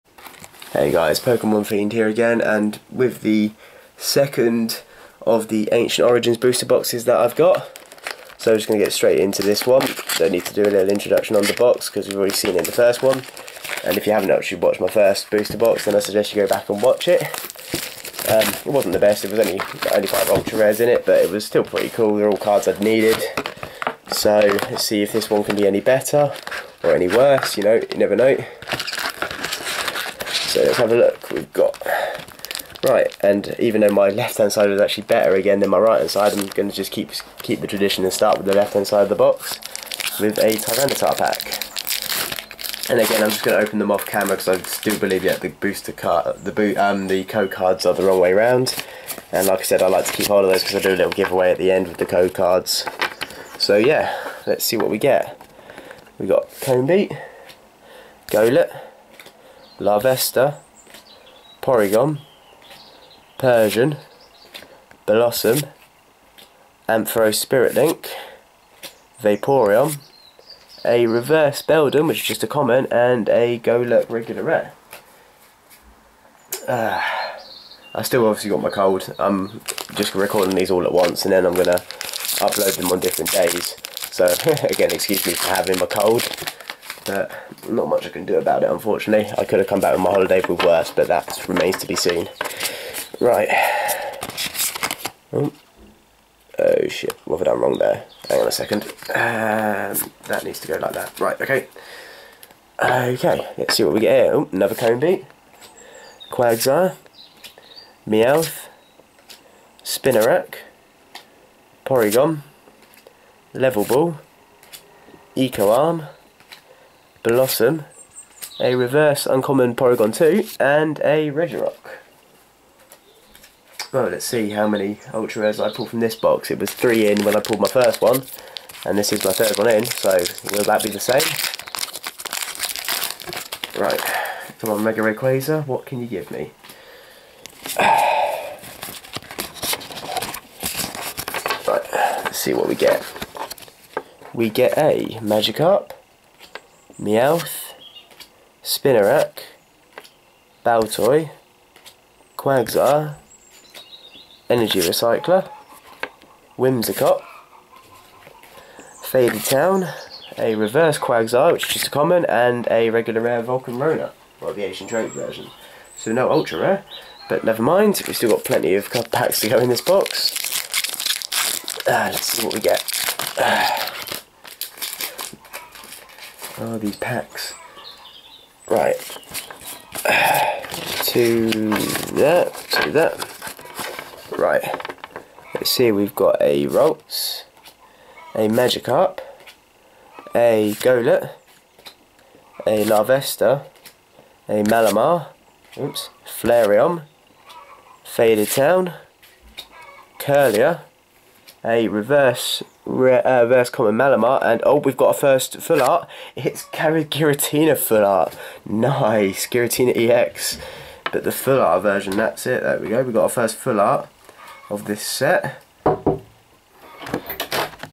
Ancient Origins Booster Box Opening! Box 2 pt.1
Right this is the first 3-4 mins of this box. Basically my computer kept fucking up every time i try to edit it so I'm just uploading both clips separately. I know its stupid but sorry i can't do much about it :(.